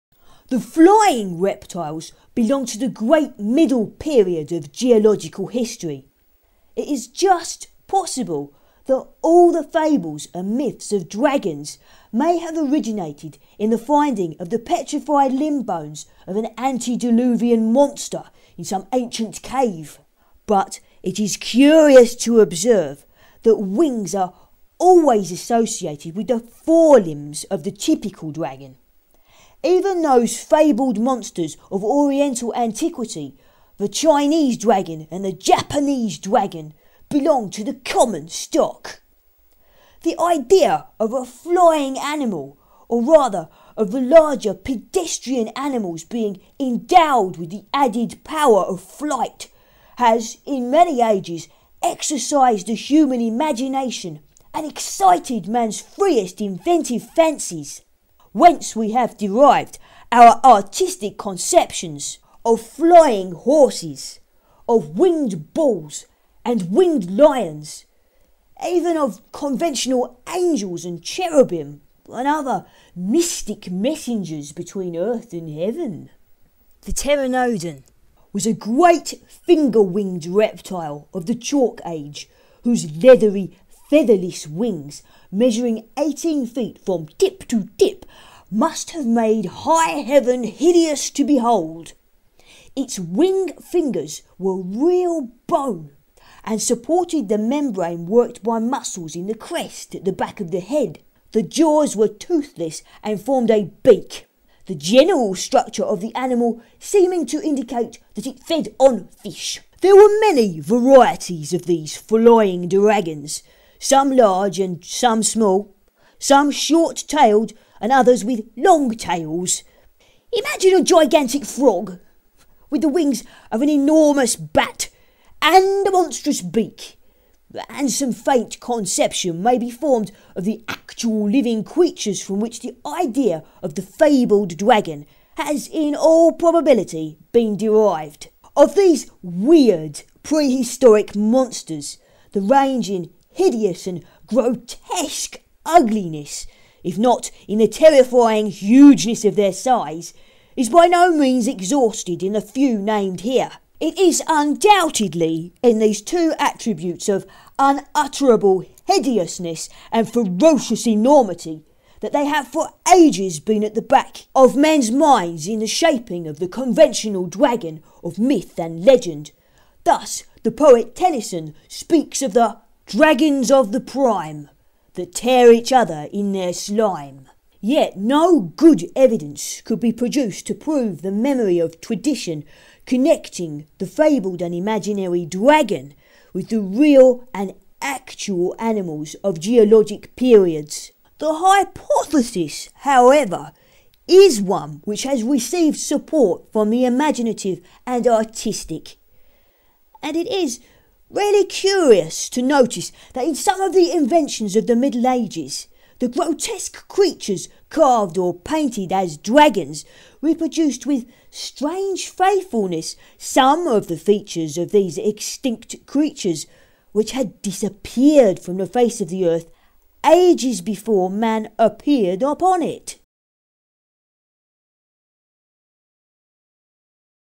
CH 8 (3/4) The Real Dragons of Nature
Third installment of the eighth and final chapter of Frederick William Hackwood's study of dragons and dragonlore, presented here to hear as an audiobook.

The flying reptiles belonged to the great middle period of geological history. It is just possible that all fables and myths of dragons may have originated in the finding of the petrified limb-bones of an antediluvian monster in some ancient cave. But it is curious to observe that wings are always associated with the forelimbs of the typical dragon. Even those fabled monsters of Oriental antiquity, the Chinese dragon and the Japanese dragon, belong to the common stock. The idea of a flying animal, or rather of the larger pedestrian animals being endowed with the added power of flight, has in many ages exercised the human imagination and excited man's freest inventive fancies ; whence we have derived our artistic conceptions of flying horses, of winged bulls and winged lions, even of conventional angels and cherubim, and other mystic messengers between earth and heaven.

The Pteranodon was a great finger-winged reptile of the Chalk Age whose leathery, featherless wings, measuring 18 feet from tip to tip, must have made high heaven hideous to behold. Its wing-fingers were real bone and supported the membrane worked by muscles in the crest at the back of the head ; the jaws were toothless and formed a beak ; the general structure of the animal seeming to indicate that it fed on fish.

There were many varieties of these flying-dragons, some large and some small, some short-tailed and others with long tails. Imagine a gigantic frog with the wings of an enormous bat and a monstrous beak, and some faint conception may be formed of the actual living creatures from which the idea of the fabled dragon has in all probability been derived.

Of these weird prehistoric monsters the range in hideous and grotesque ugliness, if not in the terrifying hugeness of their size, is by no means exhausted in the few named here.

It is undoubtedly in these twos attributes of unutterable hideousness and ferocious enormity that they have for ages been at the back of men's minds in the shaping of the conventional dragon of myth and legend. Thus the poet Tennyson speaks of the -

"Dragons of the prime,
That tare each other in their slime."

Yet no good evidence could be produced to prove the memory of tradition connecting the fabled and imaginary dragon with the real and actual animals of Geologic periods.

The hypothesis, however, is one which has received support from the imaginative and artistic. And it is really curious to notice that in some of the inventions of the Middle Ages, the grotesque creatures carved or painted as dragons reproduced with strange faithfulness some of the features of these extinct creatures which had disappeared from the face of the earth ages before man appeared upon it.